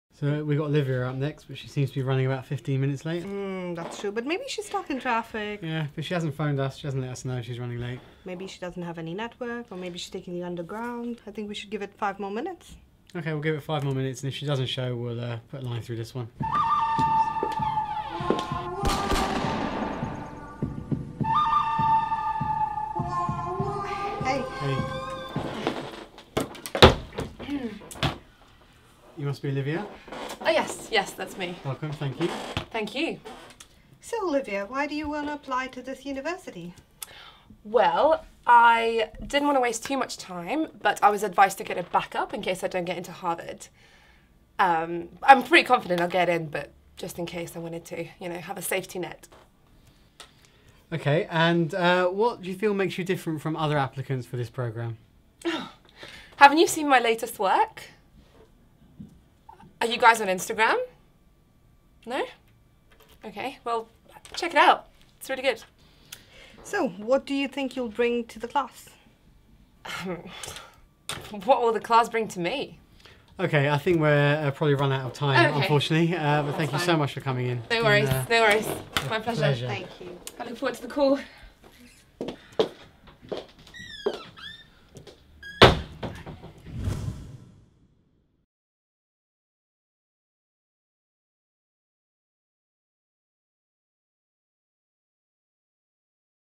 MBA Admissions Interview - The Arrogant One
When preparing for an MBA interview, it’s important to make sure you remain humble. See how uncomfortable an interview can get when the interviewee is arrogant.